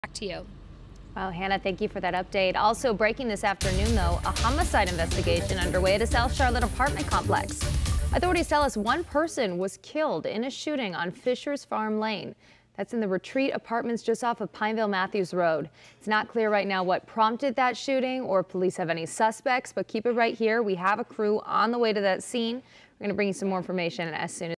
Person killed in south Charlotte shooting
Police are investigating after a person was killed in a shooting in south Charlotte Monday.